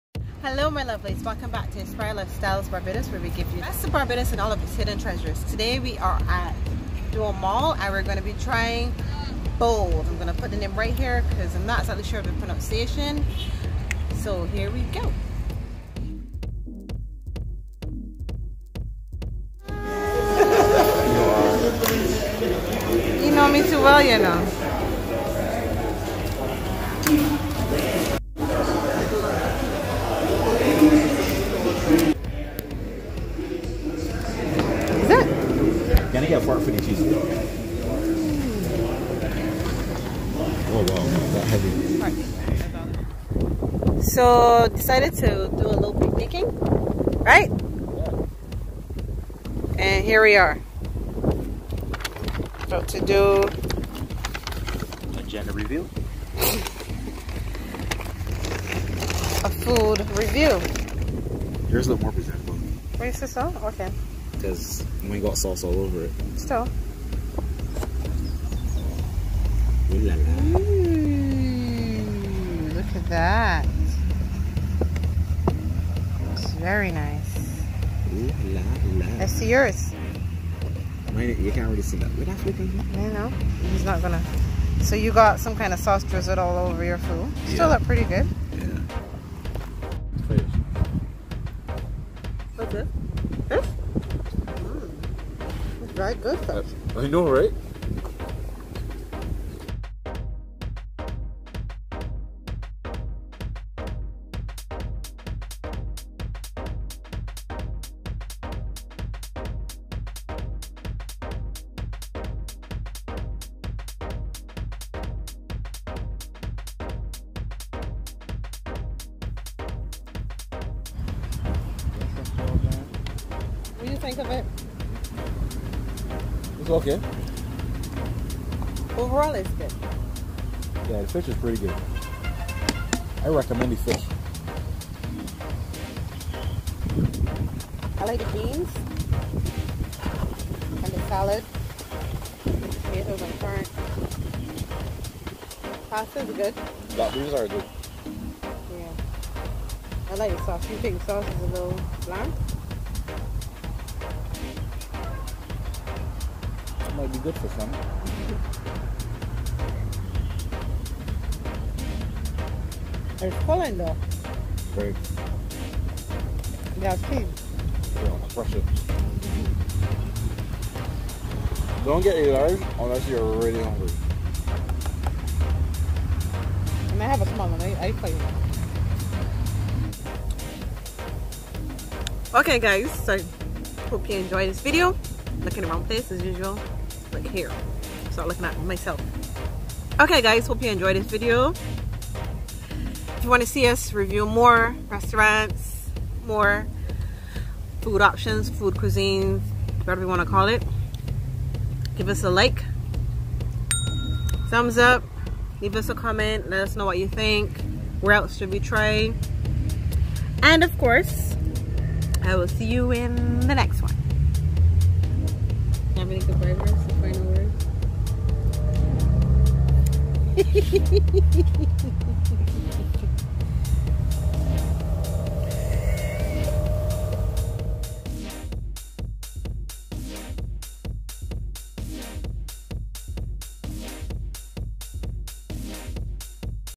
Restaurant Review- BOWL'D | Dome Mall Warrens
We love to do Restaurant Reviews 1 - because we live good food and cuisine and to give you guys a before hand idea of what to expect. BOWL'D is a combination based Restaurant with multi options. From Shirmp, Chicken and Fish. Local and International flavours.

Music wanted!!

Music wanted!!